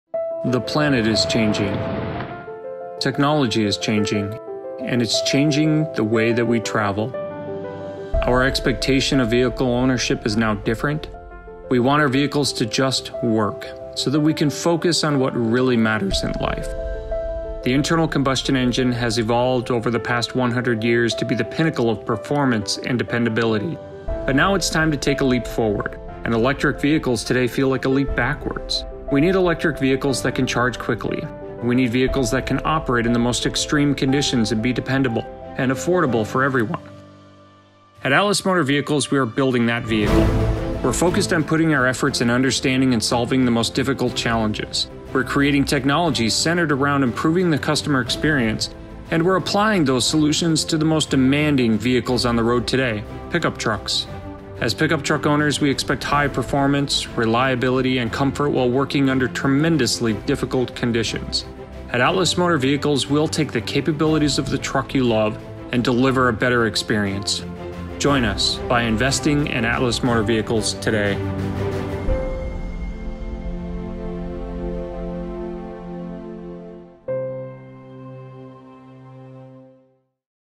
Atlis Motor Vehicles Introduction
We are in the process of developing our 100% battery electric XT pickup truck.